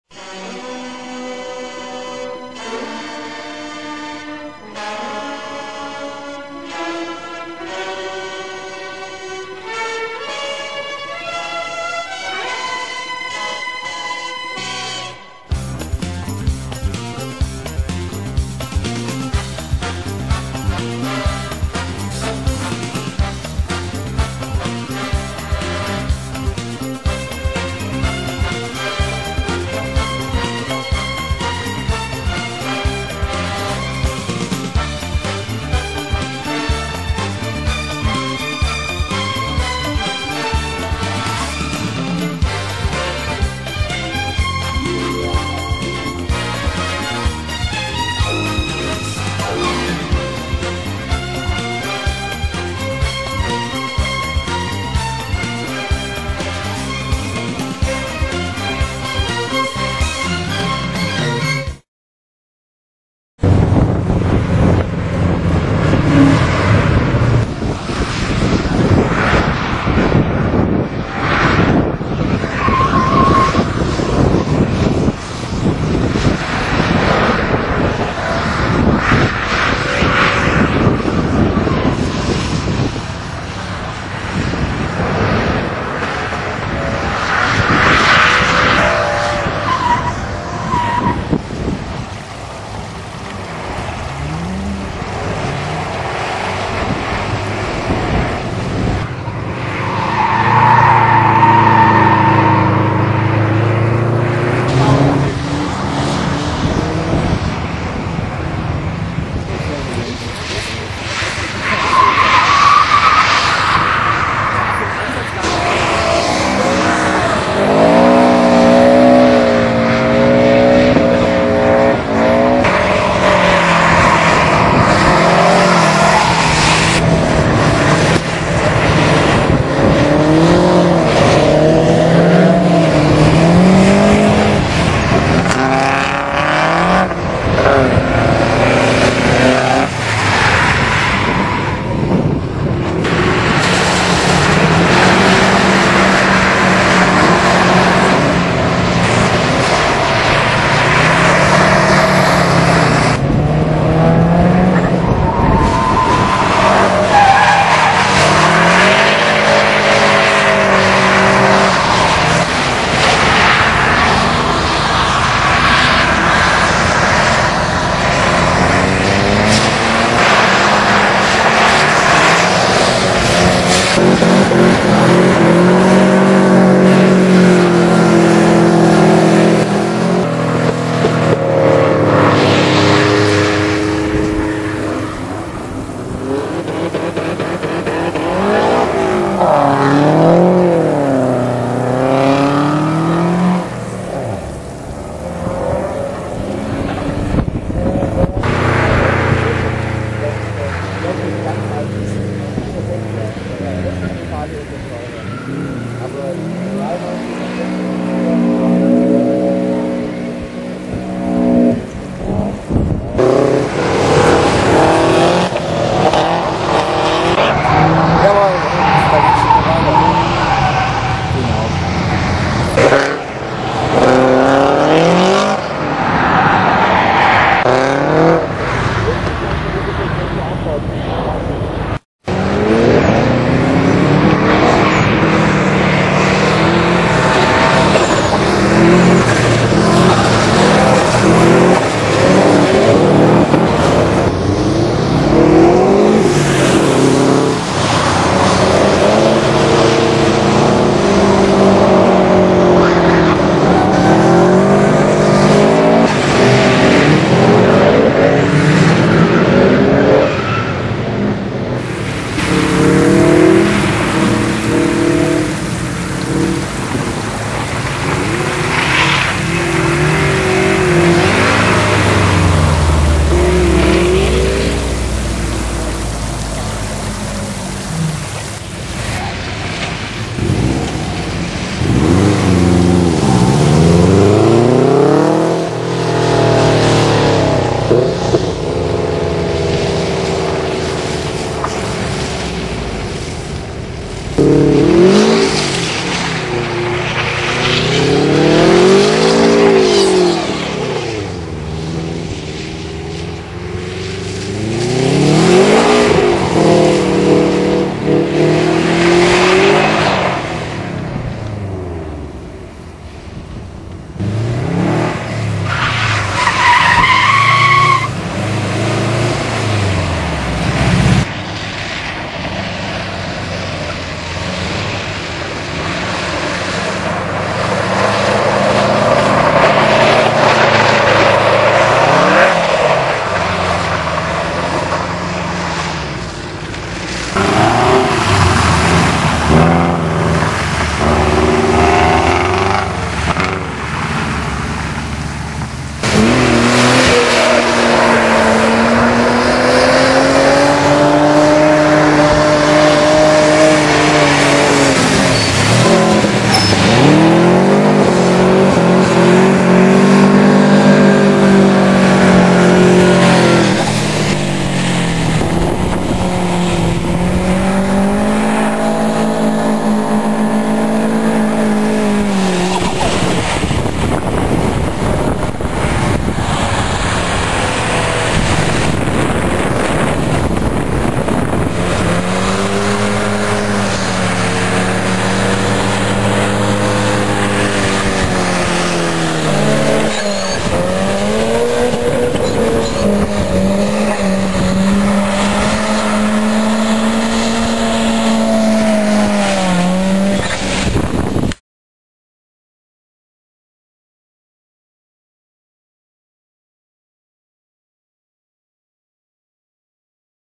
Impreza and Evo Drift Day Boxberg 2006 - Teil 2
Subaru Impreza WRX STi Mitsubishi Lancer Evo and Nissan Skyline drifting around at a German Test Track...